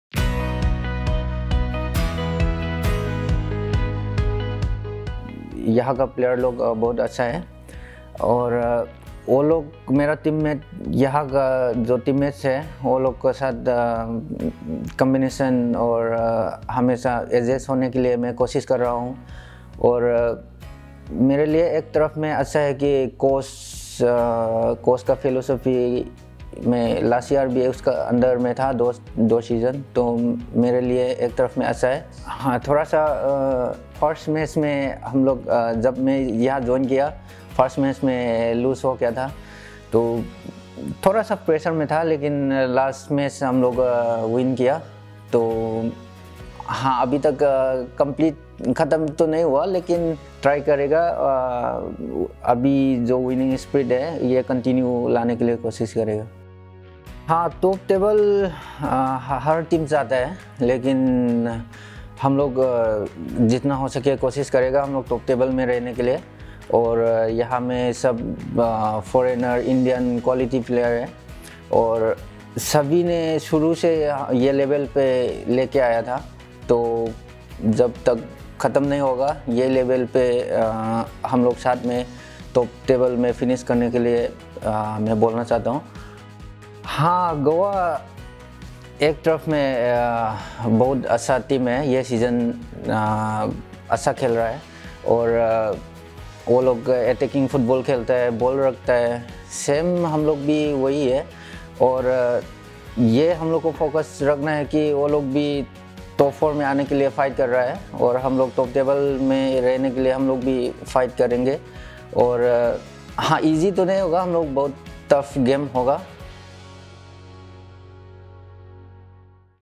In Conversation with Jackichand Singh
“We need to maintain our position until the end of the season..” 💪

🎥 | Jackichand Singh aims to keep TheIslanders at the top of the table as he previews MCFCFCG! 🗣